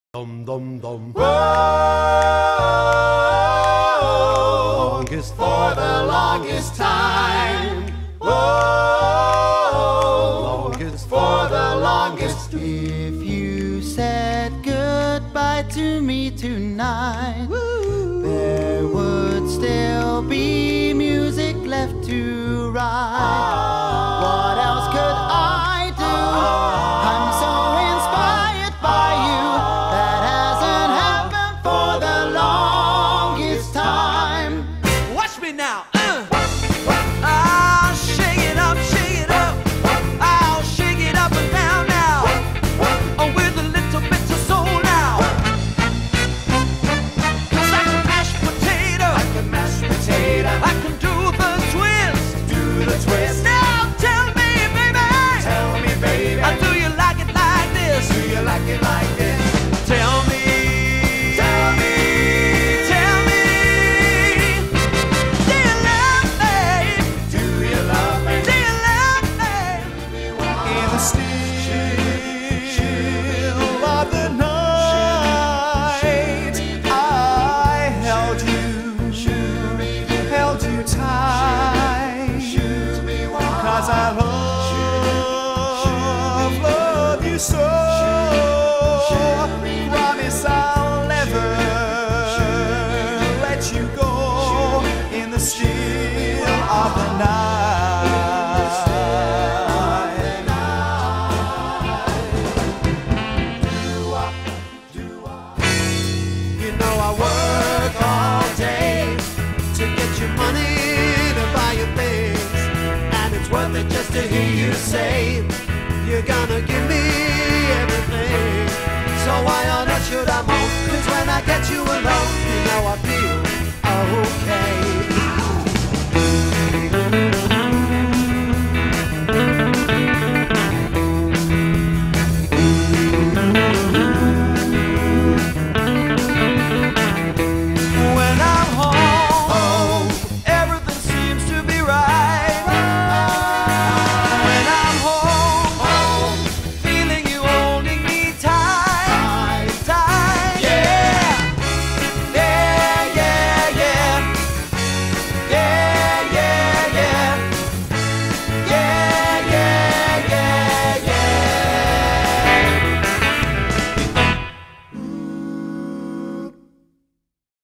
The Rockafellas
Combining some of the West End’s and British Music Industry’s best talent, the Rockafellas are an all male, close harmony group that bring the golden age of music to every event. From The Beatle’s to The Four Seasons, the Rockafellas provide something for every one, and are guaranteed to get your party rocking!